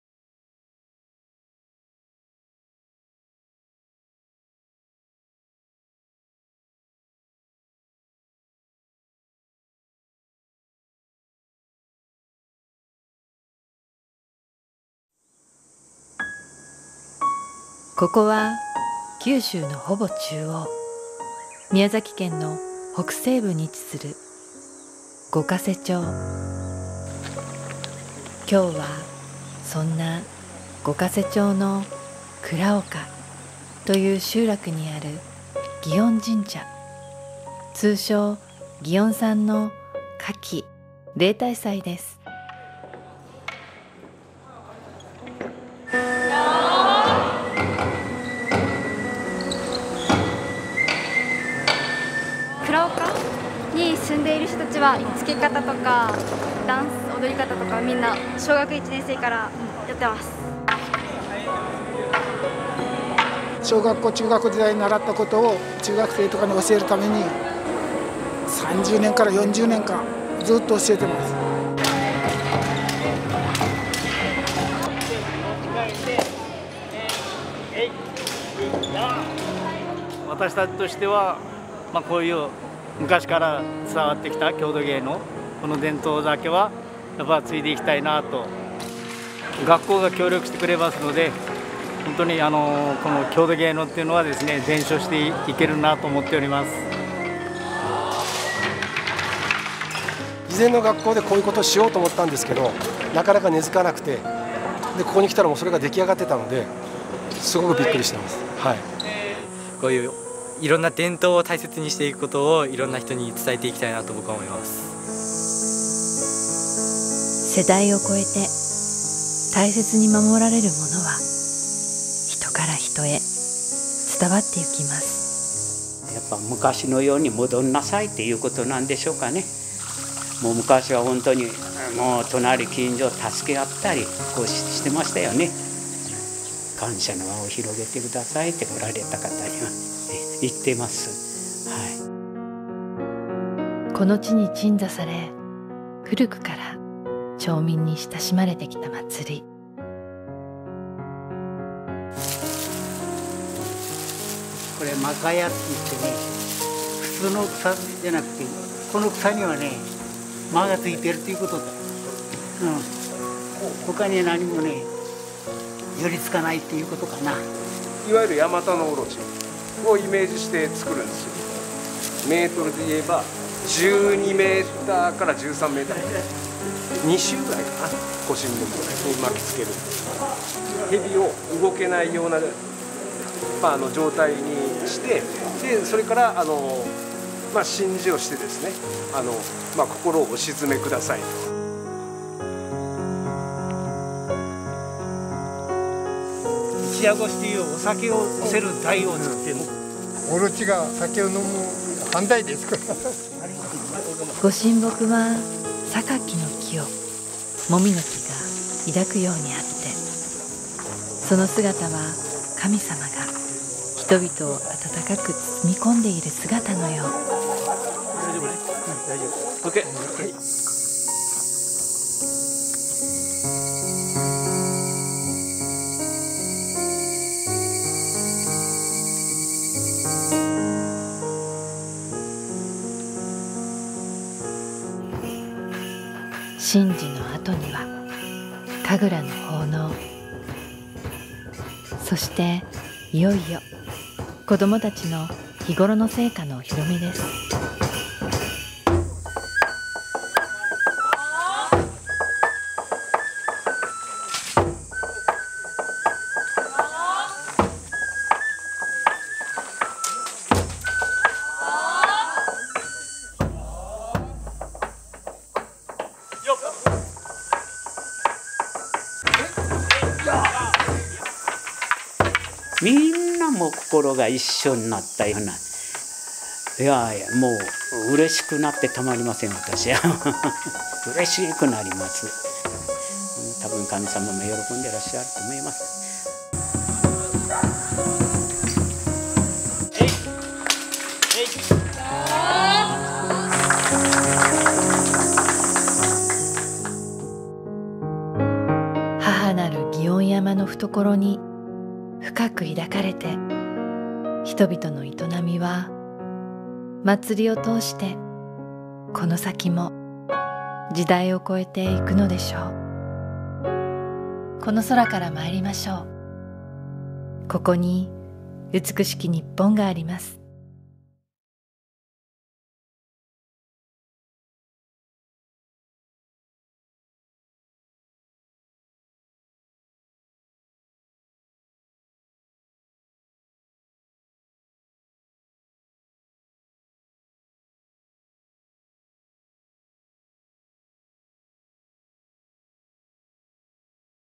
[Beautiful Japan]Miyazaki prefecture Gokase-cho ”Best trip in Japan ever”
Directed by Naomi Kawase
Japanese people believe Miyazaki prefecture was the first place where Japanese gods came down. Watch this video and enjoy beautiful Miyazaki,full of myths and legends.